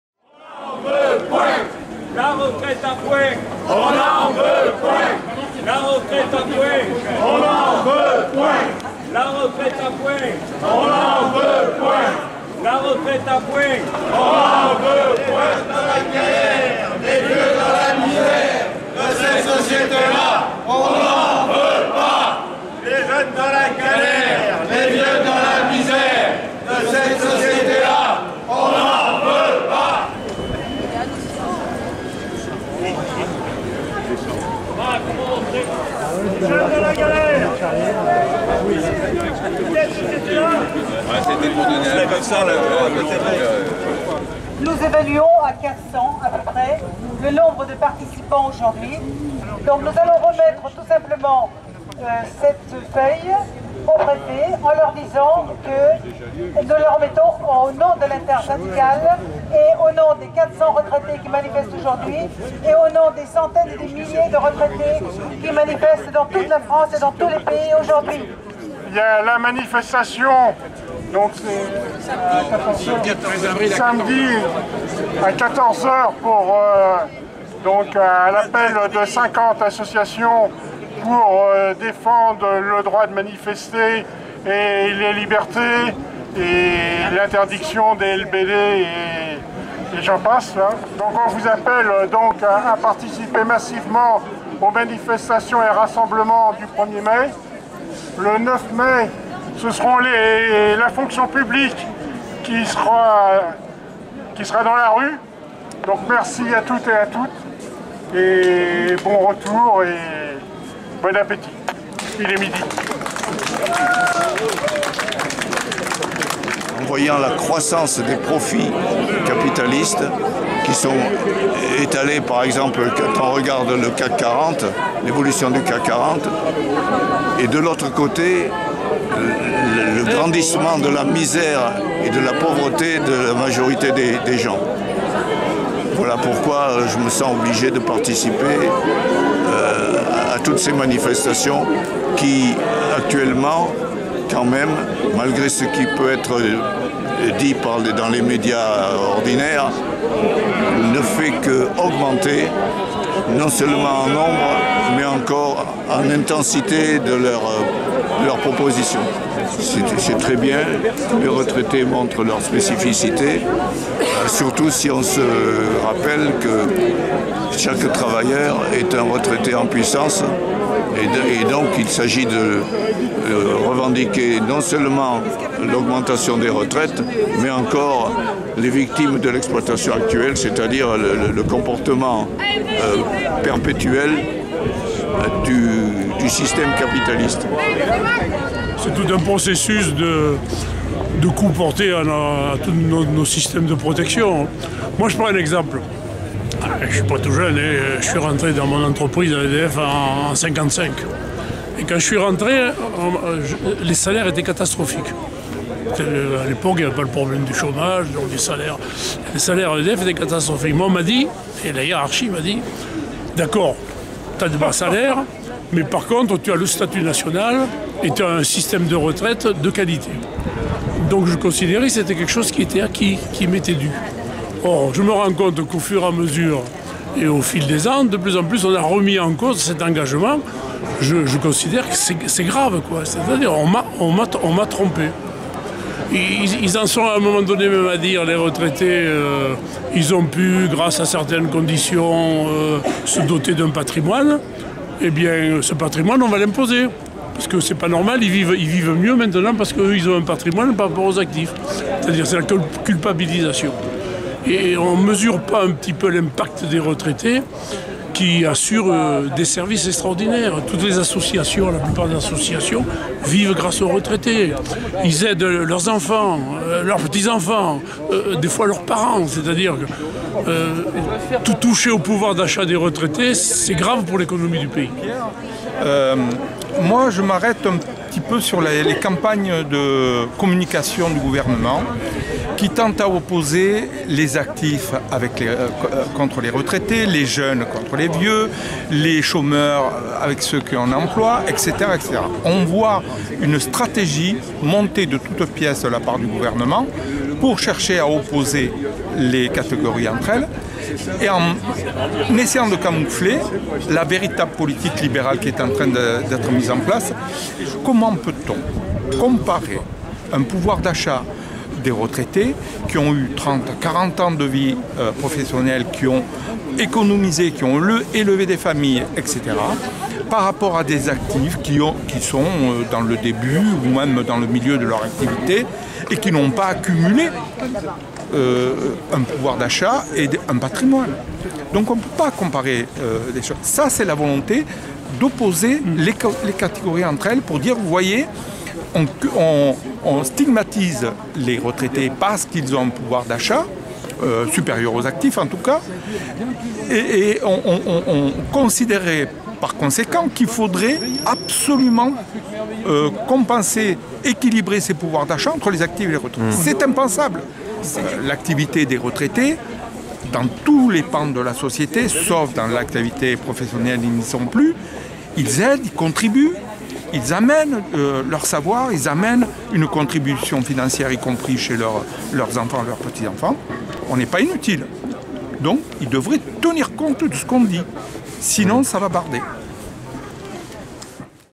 400 retraités manifestent à Montpellier contre la politique du gouvernement
utilisation des images soumise à autorisation

Environ 400 retraités se sont réunis ce jeudi 11 avril 2019 à Montpellier, pour protester contre la politique du gouvernement et notamment la hausse de la CSG qui les concerne, ou la remise en question de l'âge légal pour le départ en retraite. Même si certains d'entre eux sont des visages familiers des manifestations du samedi, l'ambiance est toute autre avec un encadrement très léger par deux fourgons de CRS, et des slogans un peu plus... répétitifs. "Les jeunes dans la galère, les vieux dans la misère, de cette société là, on n'en veut pas !"

L'occasion pour ces personnes, dont de nombreux anciens syndicalistes toujours très impliqués, de montrer leur engagement dans le mouvement social actuel et de rappeler qu'ils ne sont pas les vaches à lait de la politique fiscale. Un appel à la convergence est lancé, et à rejoindre la manifestation du samedi 13 avril, aux côtés des gilets jaunes et des associations qui ont récemment lancé un appel à manifester contre la loi dite "anti-casseurs".